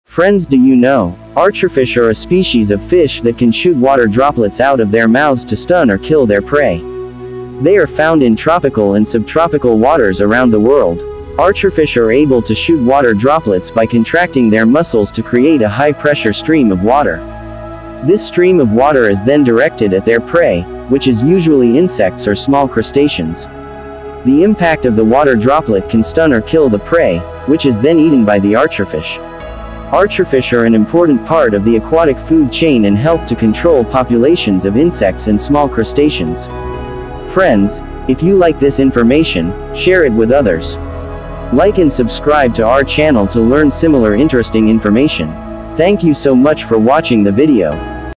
The Archer Fish: A Unique and Fascinating Predator.
Archerfish are a species of fish that can shoot water droplets out of their mouths to stun or kill their prey. They are found in tropical and subtropical waters around the world. Archerfish are able to shoot water droplets by contracting their muscles to create a high pressure stream of water. This stream of water is then directed at their prey, which is usually insects or small crustaceans. The impact of the water droplet can stun or kill the prey, which is then eaten by the archerfish. Archerfish are an important part of the aquatic food chain and help to control populations of insects and small crustaceans.